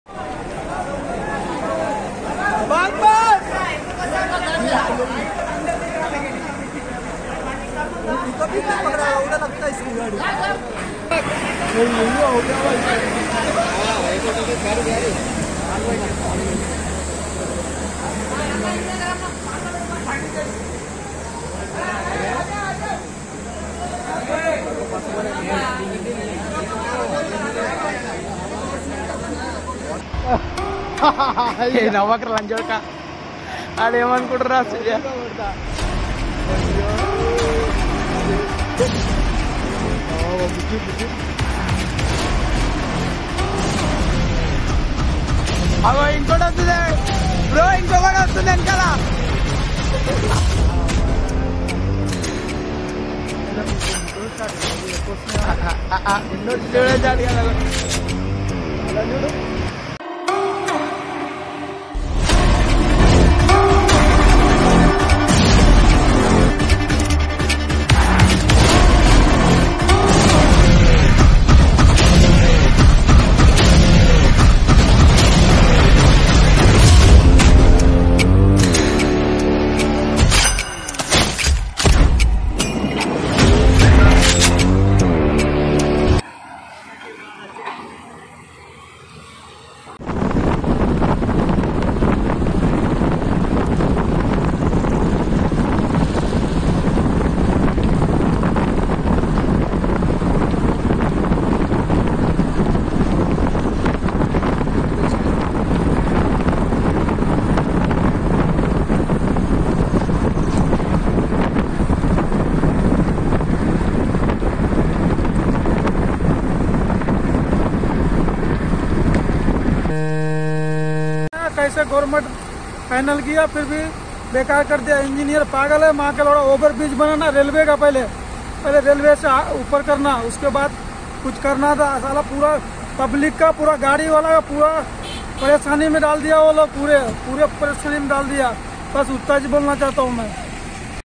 NH44 జాతీయ రహదారి రామాయంపేట దగ్గర వర్షపు నీరు బివాచం
## NH44 జాతీయ రహదారి రామాయంపేట దగ్గర రోడ్డు ఇరు వైపులా మేడ్చల్ నుండి మనోరాబాద్ టోల్గెటు వరకు Traffic అంతరాయం కలిగిoధీ